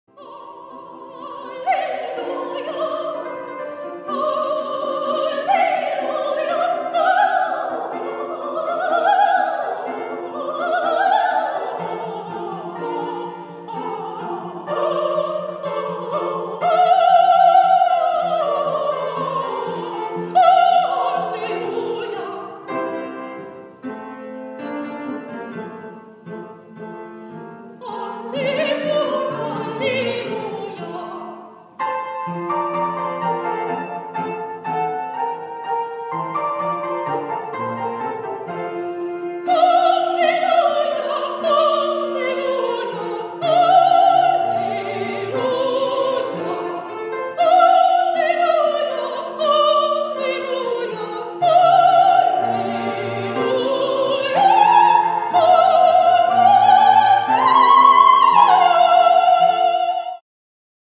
THROUGH GRIEF TO HOPE– A CONCERT
September 26 – 6th & I*

IN Series’ opening weekend festival culminates in a concert about finding light after darkness. Beloved sacred works of Bernstein and Mozart are paired with poetry offered especially for this concert by nine acclaimed poets from around the world, including Pulitzer Prize winner Forest Gander, Nobel nominee Raul Zurita, and National Book Award winner Don Mee Choi. As these poems tell of the experiences of 2020, this concert, presented by seven extraordinary vocalists, moves from lamentation to hope and renewal. Part concert, part theater, part worship, this program memorializes the losses of the past year and turns our gaze to a new beginning. With sopranos Teresa Ferrara, Melissa Wimbish, and Judy Yannini; mezzo-soprano Elizabeth Mondragon; countertenor Daniel Moody; tenor Robert Chafin; baritone Carl Dupont; Noah Mitchel as the narrator; and Hui-Chuan Chen on the piano.